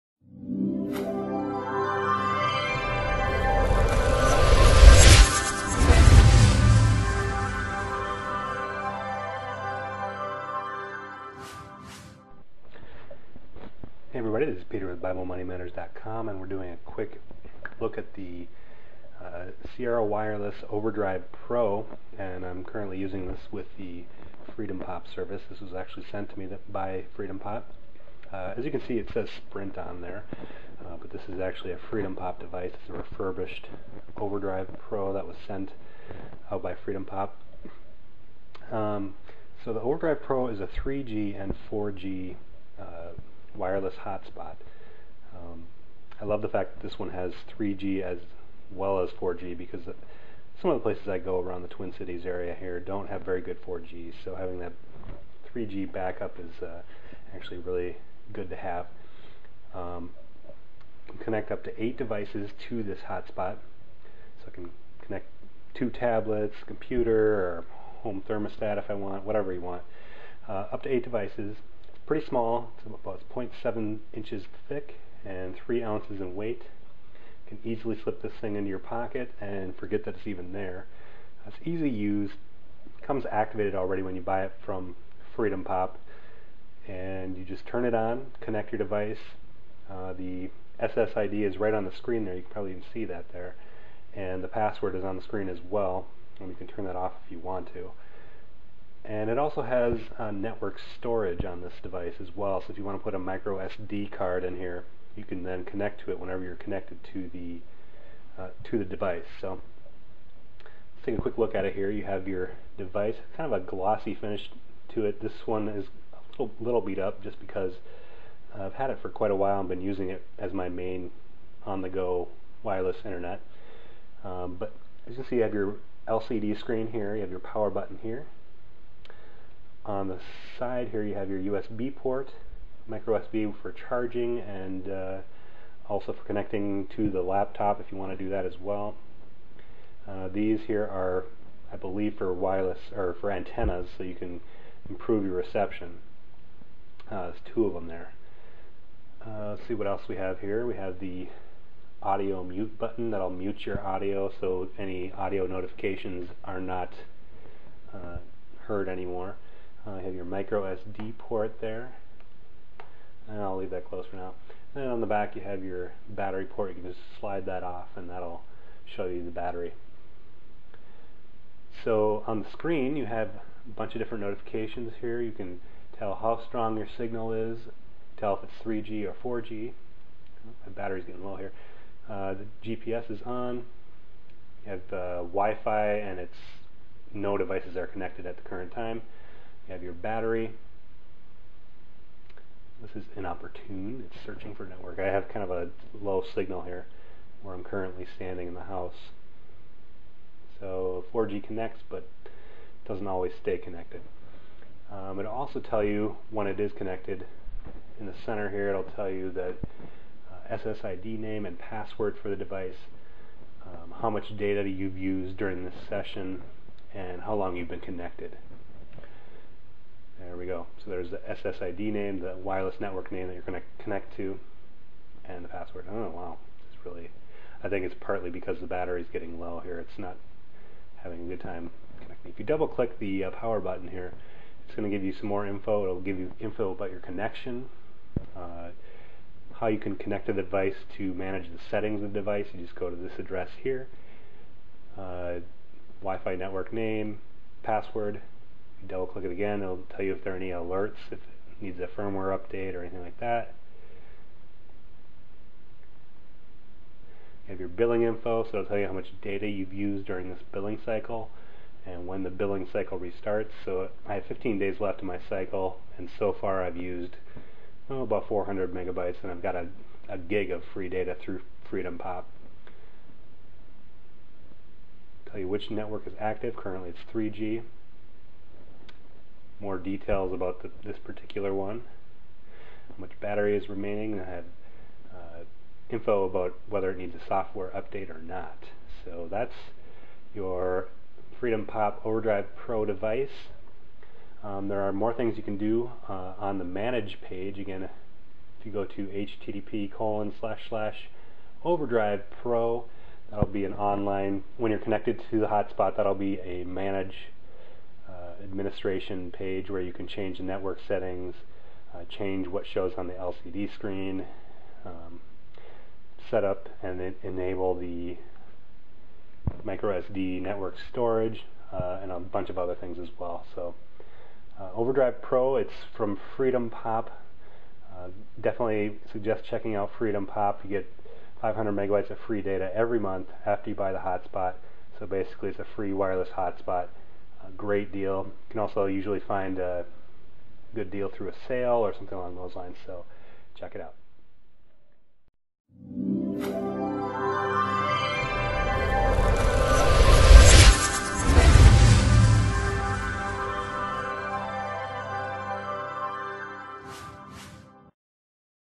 FreedomPop Freedom Spot Overdrive Pro Review: Wireless Hotspot With Free 3G/4G Internet
The FreedomPop Freedom Spot Overdrive Pro is one of the best wireless hotspots available for the free FreedomPop service currently.

- It has both 3G and 4G connectivity.
- You can connect up to 8 devices.
- Small and compact - .7 inches tall and 2.67" on each side.  Around 3 ounces.
- Has microSD port for network storage.

The device works great in my area, and I was able to get in excess of 7 Mbps download using the device.

Read a full text review of the FreedomPop Freedom Spot Overdrive Pro